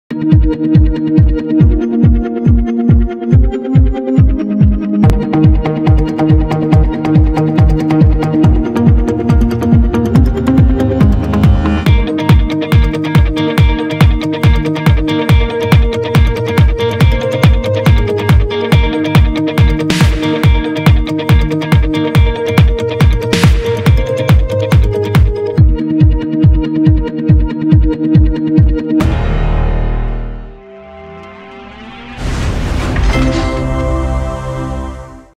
Get the Answer—Faster—with Immediate & Intuitive Signal Analysis
Engineering is all about connecting ideas and solving problems. That’s what drives the X-Series signal analyzers. They put you closer to the answer—faster—by easily linking cause and effect. See for yourself: our X-Series videos highlight the multi-touch interface that streamlines measurements and makes analysis more immediate and intuitive.